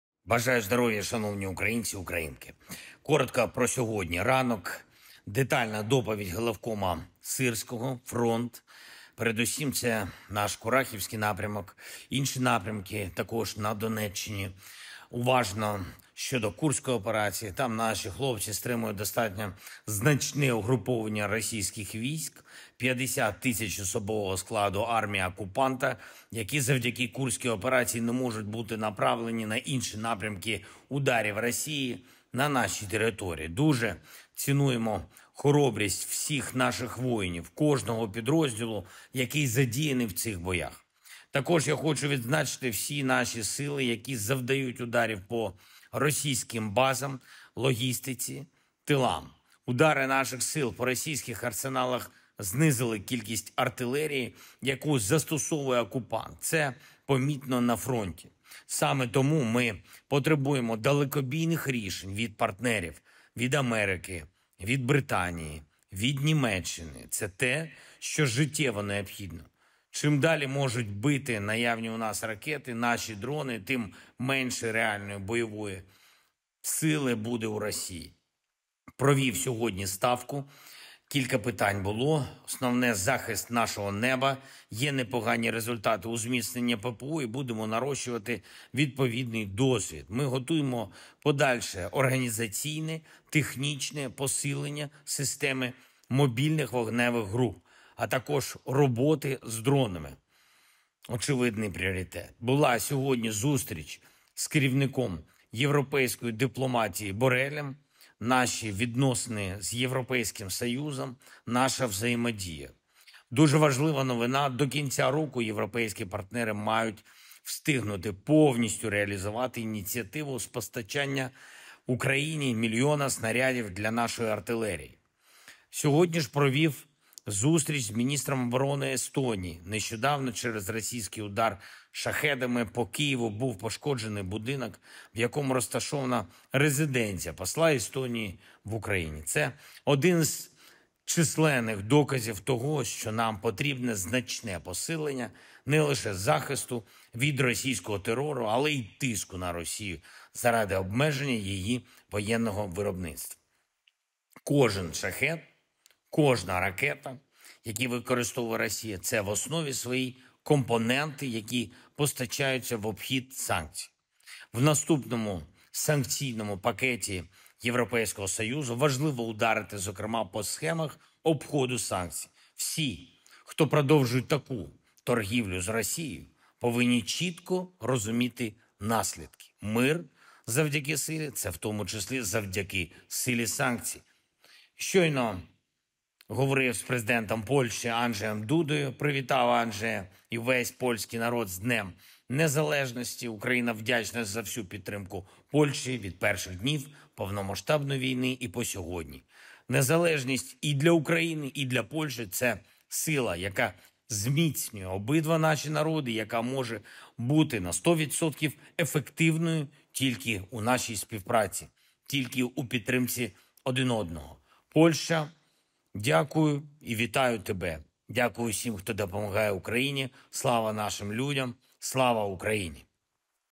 Бійці стримують значне угруповування російських військ – 50 тисяч особового складу армії окупанта
«Детальна доповідь Головкома Сирського. Фронт, передусім це наш Курахівський напрямок, інші напрямки також на Донеччині. Уважно – щодо Курської операції.

Там наші хлопці стримують достатньо значне угруповування російських військ – 50 тисяч особового складу армії окупанта, які завдяки Курській операції не можуть бути направлені на інші напрямки ударів Росії на нашій території. Дуже цінуємо хоробрість усіх наших воїнів, кожного підрозділу, який задіяний у цих боях.

Також хочу відзначити всі наші сили, які завдають ударів по російським базам, логістиці, тилам. Удари наших сил по російських арсеналах знизили кількість артилерії, яку застосовує окупант, і це помітно на фронті. Саме тому ми потребуємо далекобійних рішень від партнерів – від Америки, від Британії, від Німеччини. Це те, що життєво необхідно. Чим далі можуть бити наявні в нас ракети, наші дрони, тим менше реальної бойової сили буде в Росії».